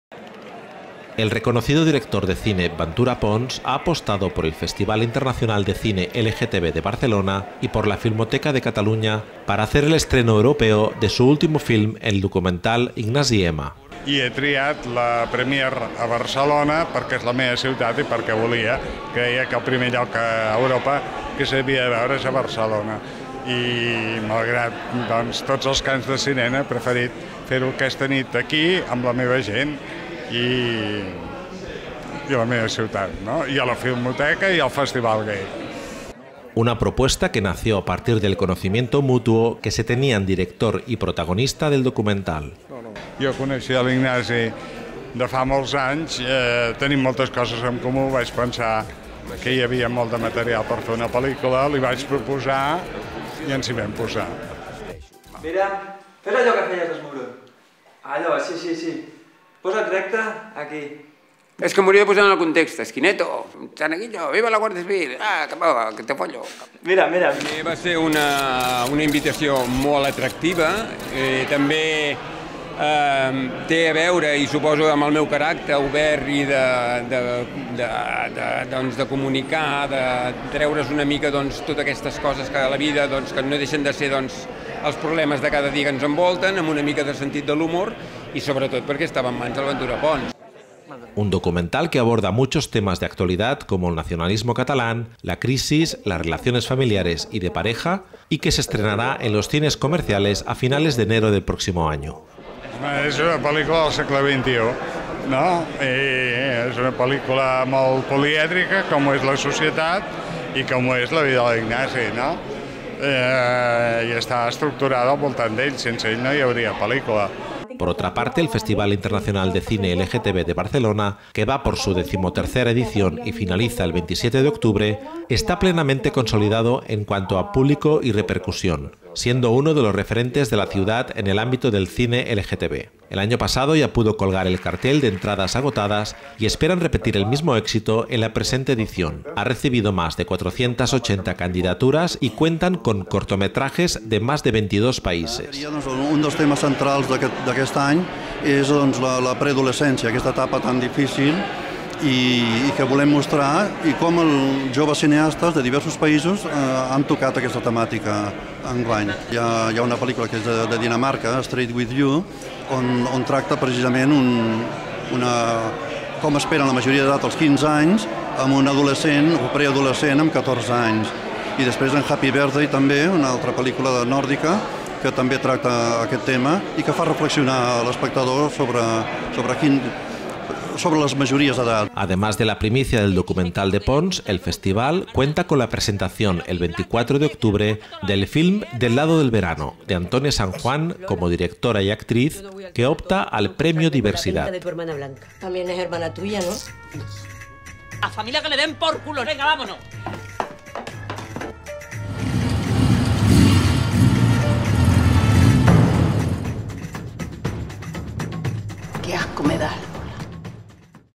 Festival Internacional de Cine LGTB en Barcelona [esp]
Subtitulat per sords, y en español.

Sigue todos nuestros programas en idemtv.com | Segueix els nostres programes a idemtv.com

El reconocido director de cine Ventura Pons ha apostado por el Festival Internacional de Cine LGTB de Barcelona y por la Filmoteca de Catalunya para hacer el estreno europeo de su último filme, el documental Ignasi M.

Una propuesta que nació a partir del conocimiento mutuo que se tenían director y protagonista del documental .

Un documental que aborda muchos temas de actualidad como el nacionalismo catalán, la crisis, las relaciones familiares y de pareja, y que se estrenará en los cines comerciales a finales de enero del próximo año.

Por otra parte , el Festival Internacional de Cine LGTB de Barcelona, ​​que va por su decimotercera edición y finaliza el 27 de octubre, está plenamente consolidado en cuanto a público y repercusión, siendo uno de los referentes de la ciudad en el ámbito de cine LGTB. El año pasado ya pudo colgar el cartel de entradas agotadas y esperan repetir el mismo éxito en la presente edición. Ha recibido más de 480 candidaturas y cuentan con cortometrajes de más de 22 países .

Además de la primicia del documental de Pons, el Festival cuenta con la presentación el 24 de octubre del film "Del lado del verano" de Antonia San Juan como directora y actriz que opta al Premio Diversidad.